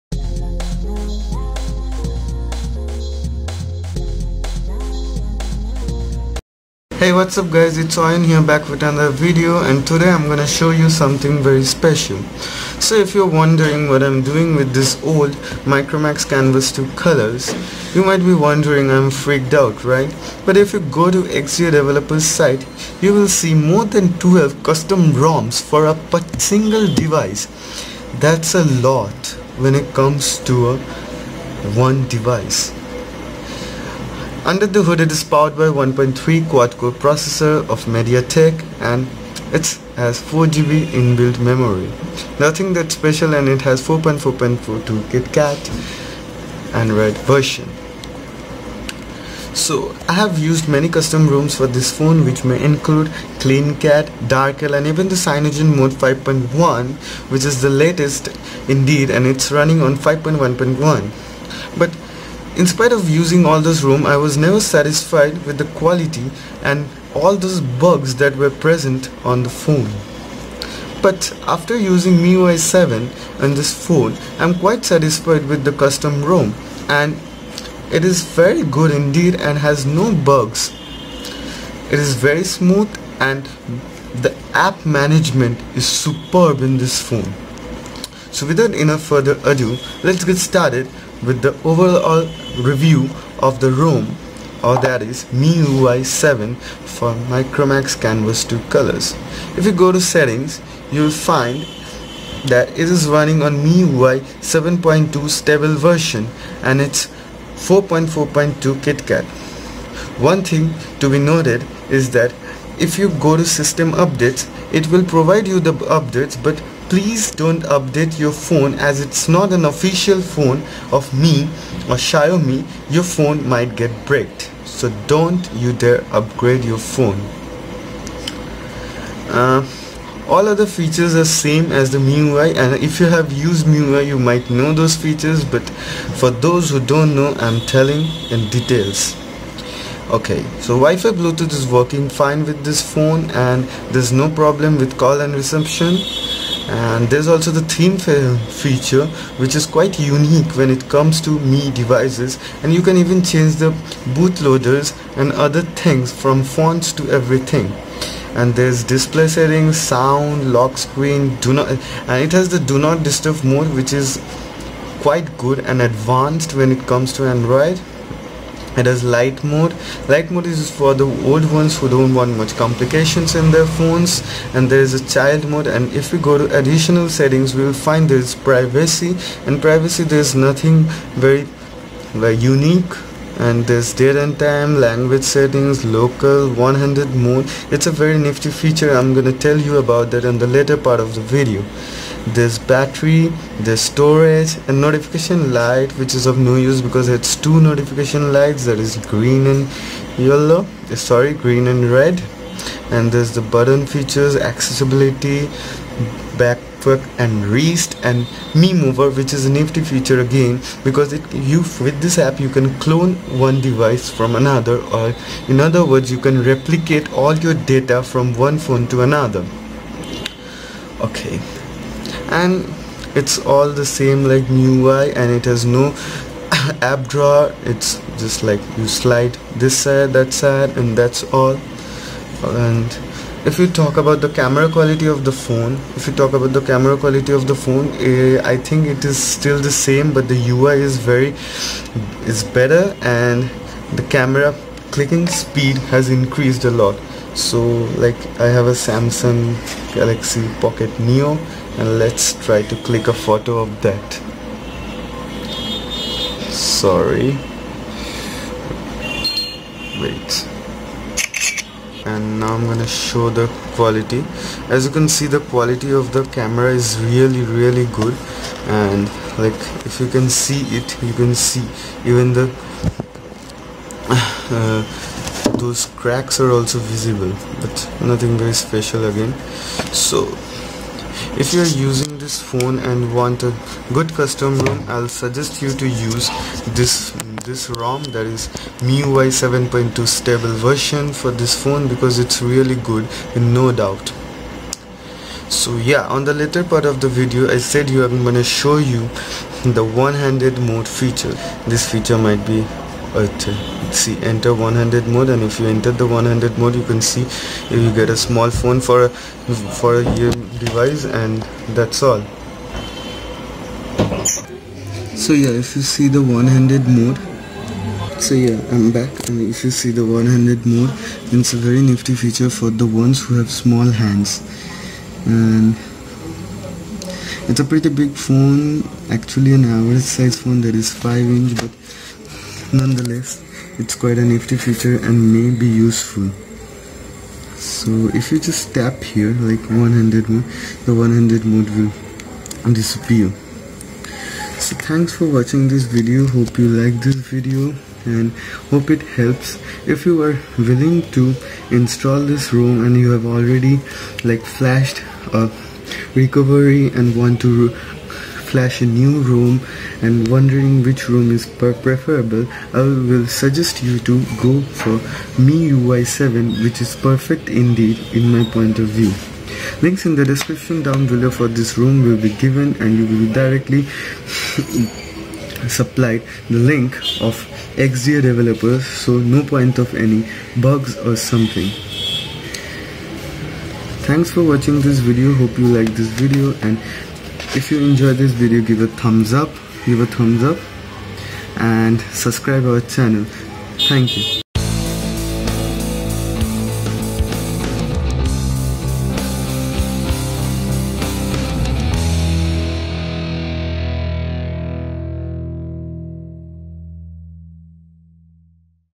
MIUI 7.2 For Micromax Canvas 2 Colours
This video is an in depth review of the MIUI 7.2 for canvas 2 colours .